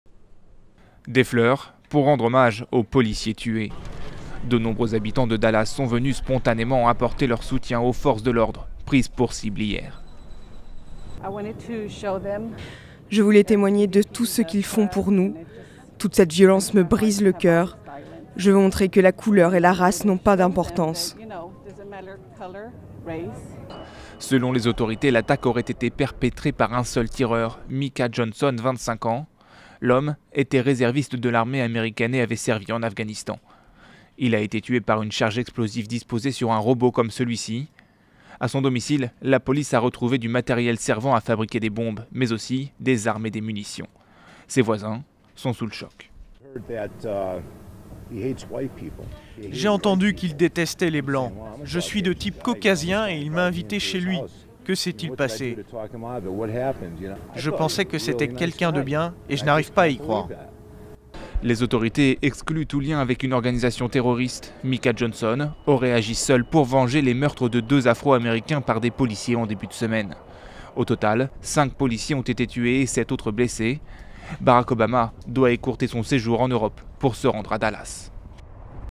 Dallas sous le choc après le meurtre de cinq policiers
De nombreux habitants de Dallas sont venus spontanément apporter leur soutien aux forces de l'ordre prises pour cible hier. De leur côté, les enquêteurs progressent. Selon eux, l'attaque aurait été perpétrée par un seul tireur : un homme de 25 ans, réserviste de l'armée et qui a servi en Afghanistan. À son domicile, la police a retrouvé du matériel servant à fabriquer des bombes, ainsi que des armes et des munitions.